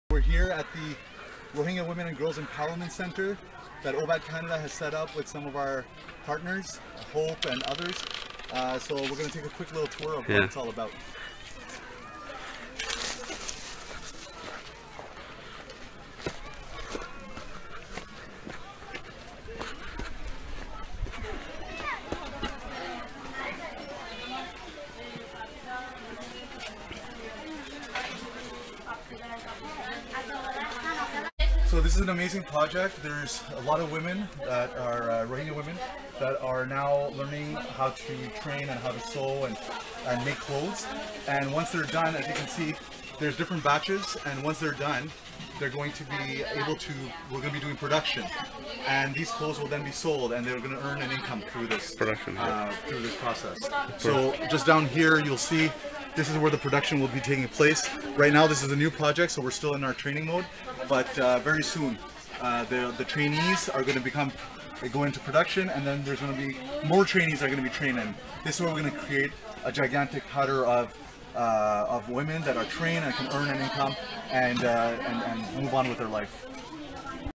OBAT's Women & Girls' Empowerment Center in Rohingya Camps
Rohingya women possess unparalleled strength and resilience after the unspeakable horrors so many have experienced. An area of key focus for OBAT is to facilitate socio-economic opportunities for them.

Dr. Shujaat Wasty (Founder and President of OBAT Canada, and Vice-President of Obat Helpers USA) takes a close look at OBAT's Women & Girls Empowerment Center that offers training to Rohingya women - including widows - to help them earn income and support their families.